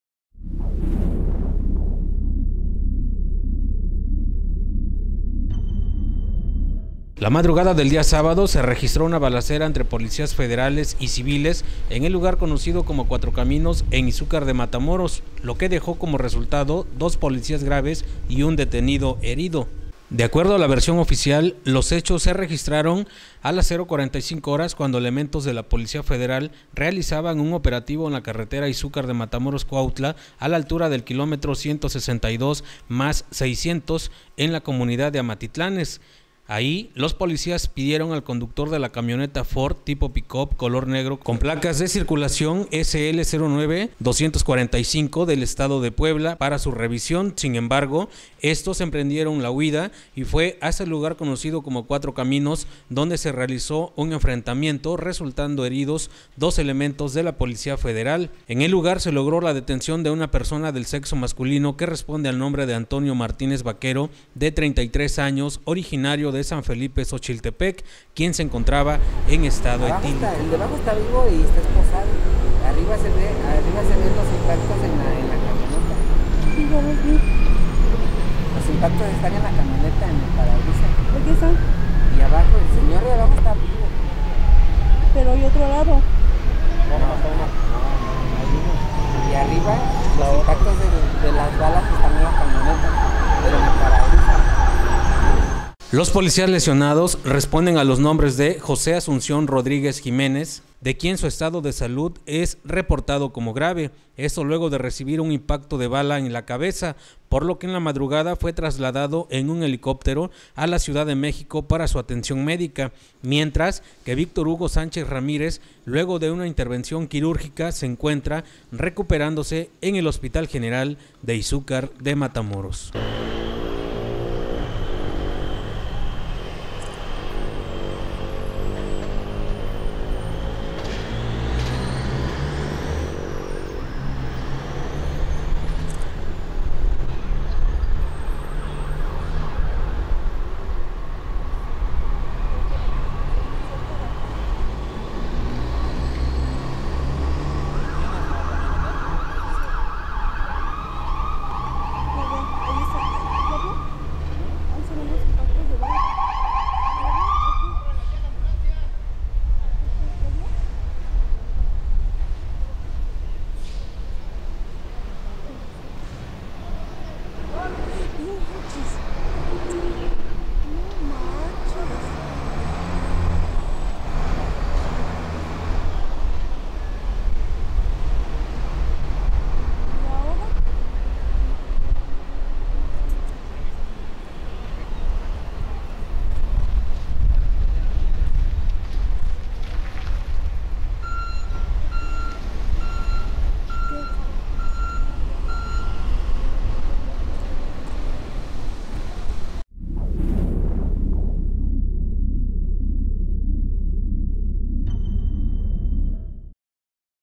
Balacera entre federales y civiles en Cuatro Caminos de Izúcar
La madrugada del día sábado se registró una balacera entre policías federales y civiles, en lugar conocido como Cuatro Caminos en Izúcar de Matamoros, lo que dejó como resultado dos policías graves y un detenido herido.

IMÁGENES EXCLUSIVAS:
*Fotografía de los policías federales lesionados
*Momento en el que capturan al presunto delincuente
*Acordonamiento de la zona frente a Sagarpa por parte de los Policías Federales
*Traslado del lesionado al Hospital General de Izúcar de Matamoros